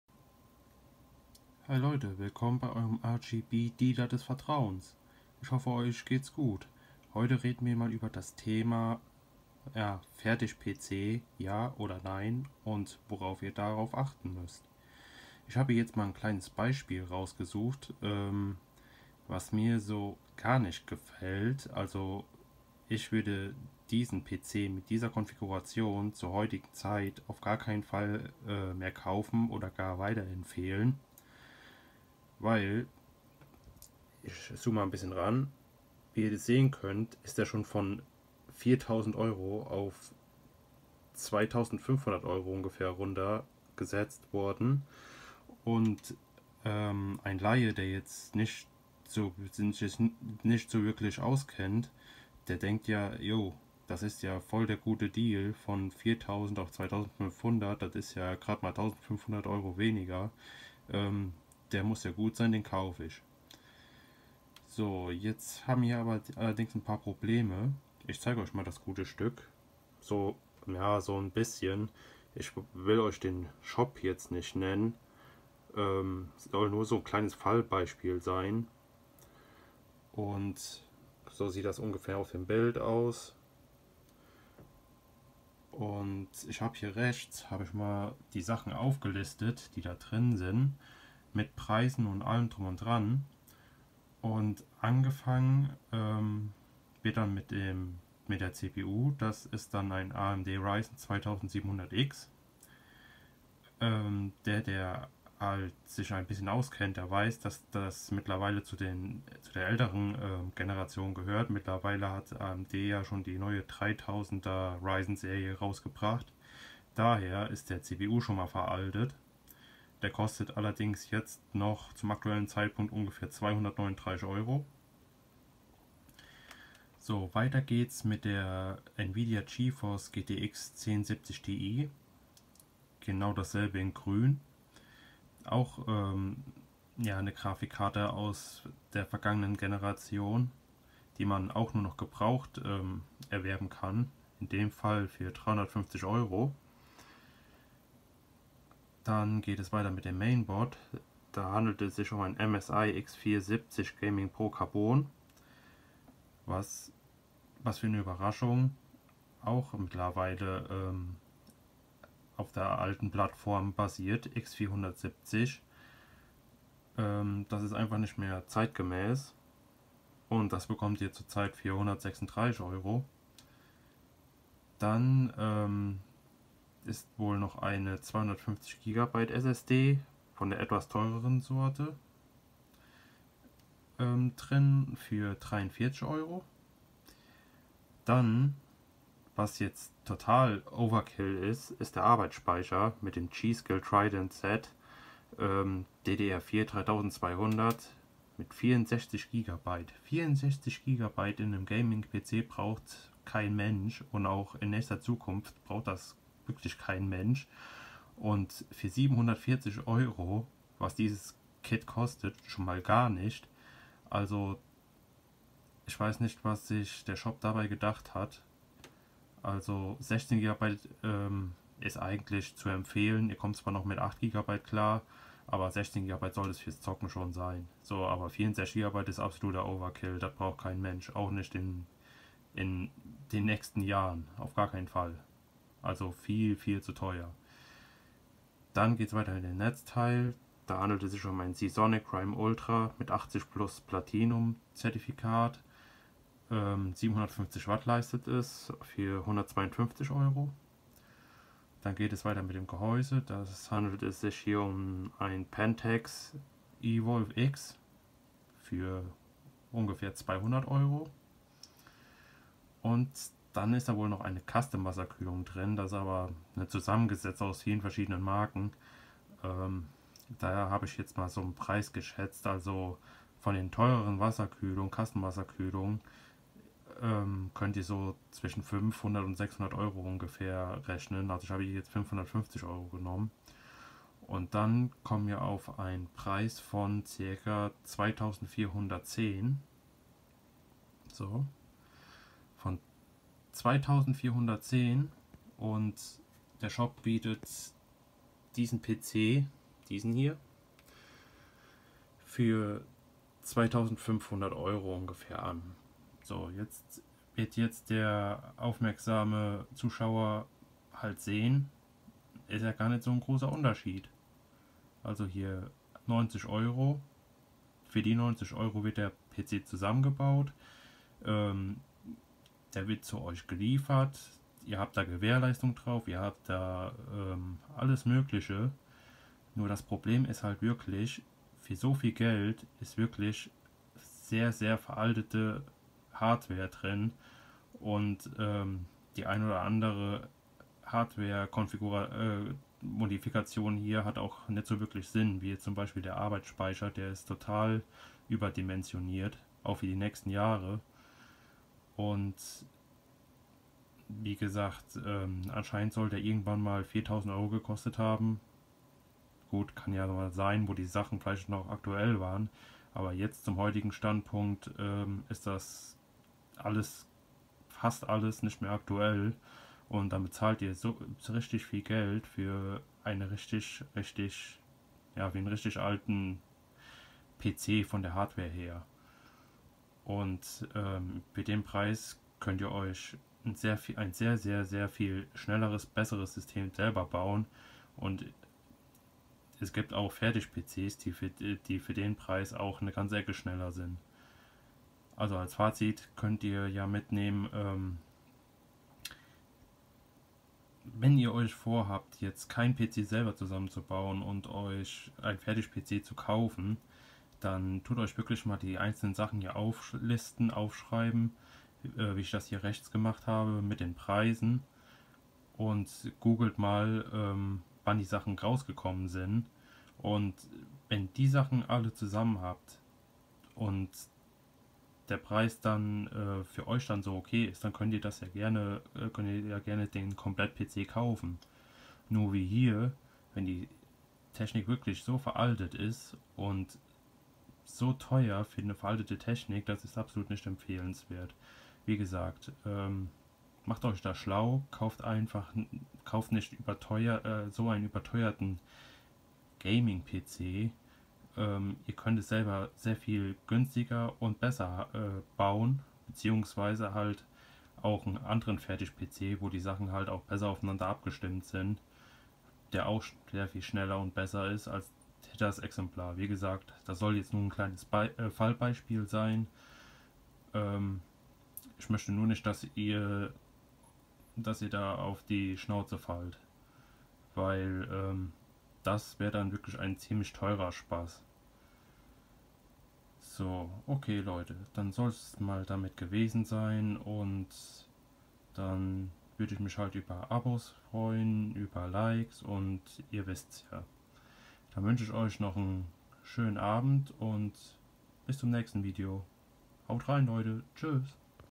Augen auf beim Fertig PC Kauf 👀 👀
Werbevideo

Servus,

Mit diesem Video zeige ich euch auf was Ihr beim Kauf eines Fertig PC’s achten müsst! Mit negativ Beispiel! Viel Spaß🙏